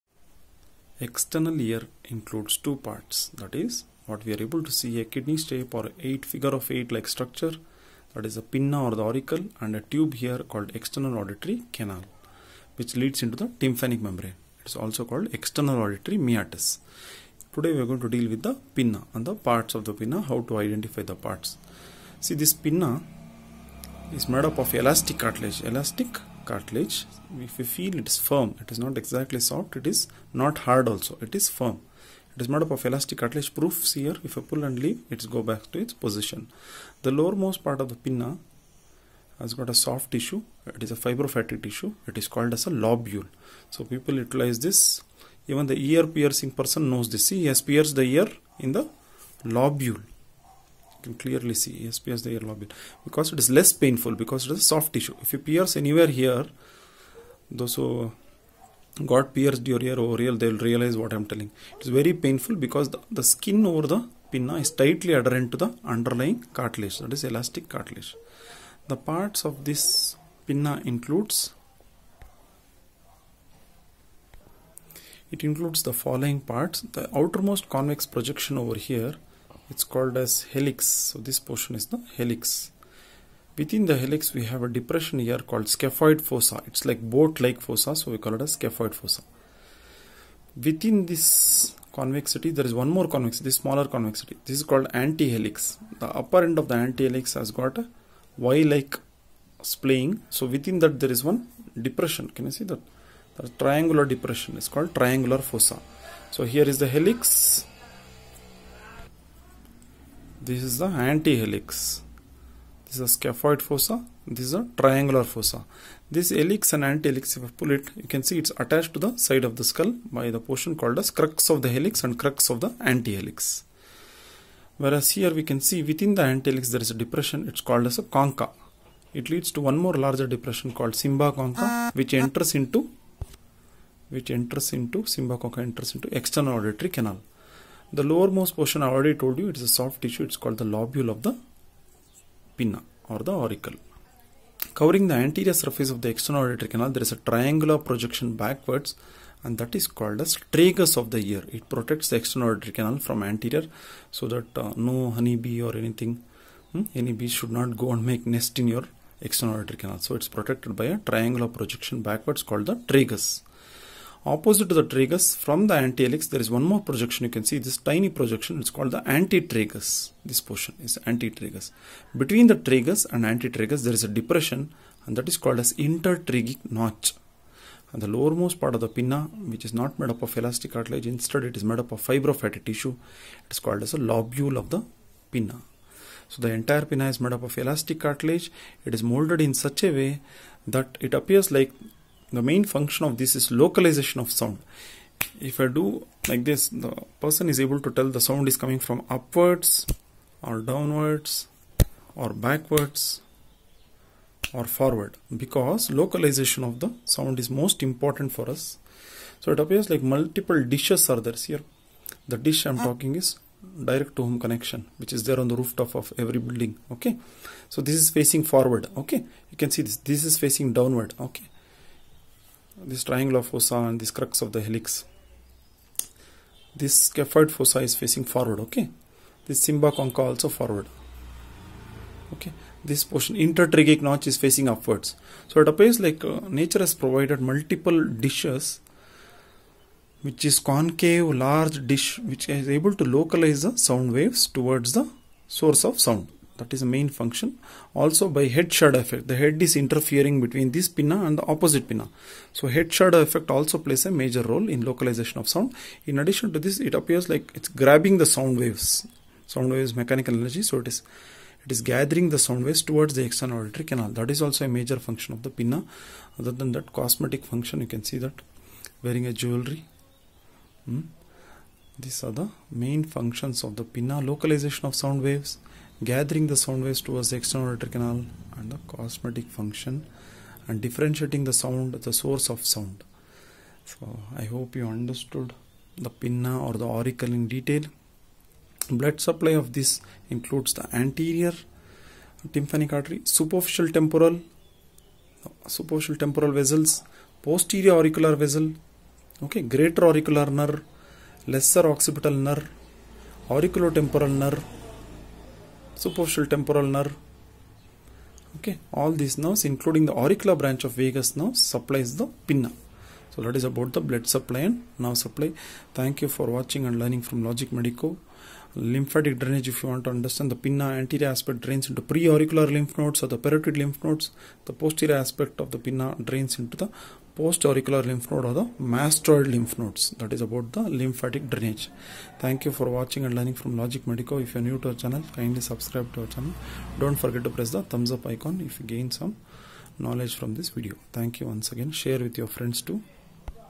External Ear| Helix| Antihelix| Tragus| Antitragus| Cymba concha | scaphoid fossa | Triangular fossa
Parts of external ear explained in detail.
Helix, antihelix, concha, cymba concha, tragus, antitragus, intertragic notch.